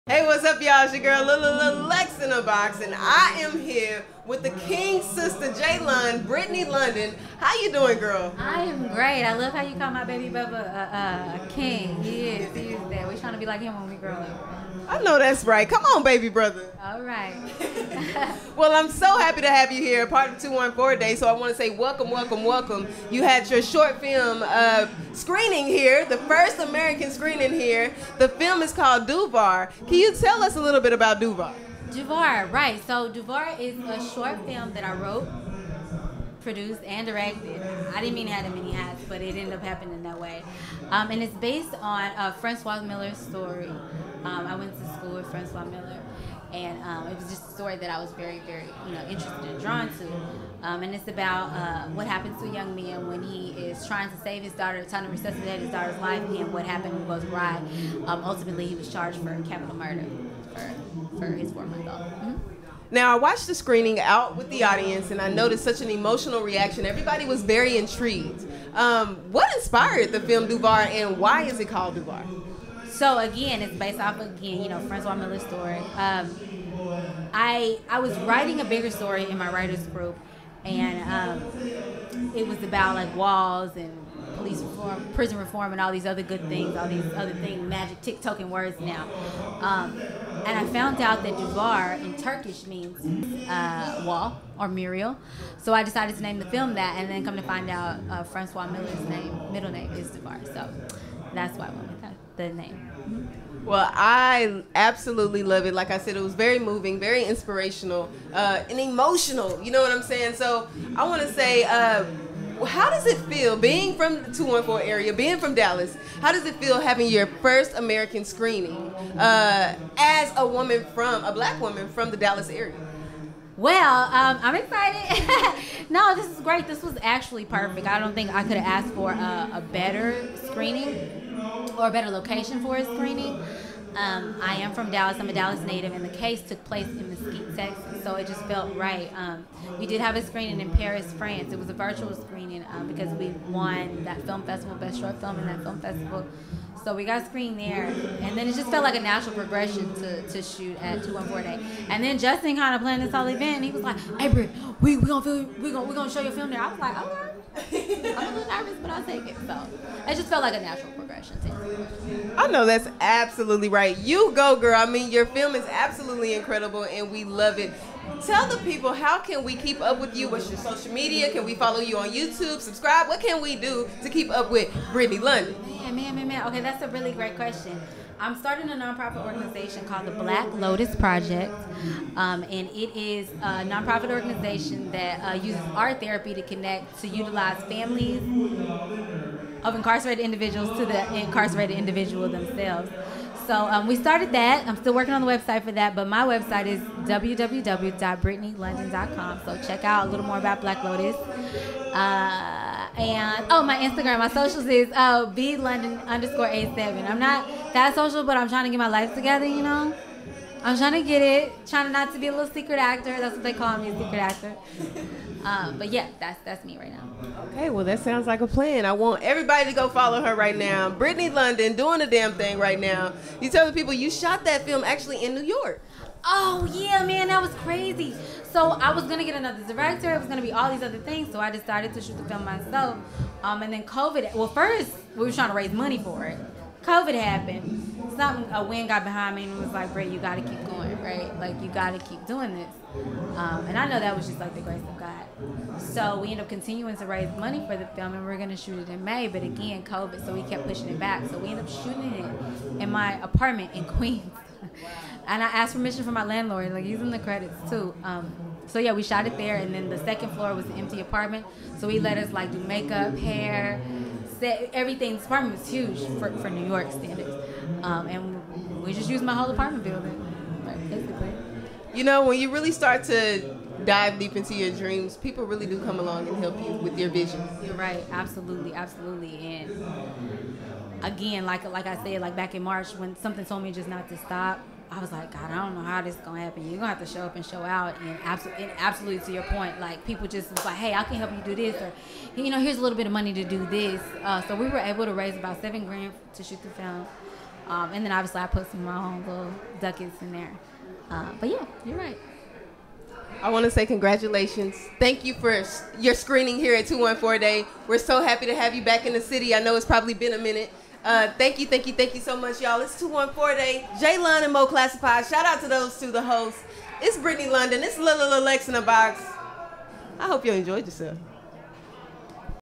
214 DAY| "DUVAR" Short Film
Director, Actress, Actor Brittney London chats about her short film, DUVAR.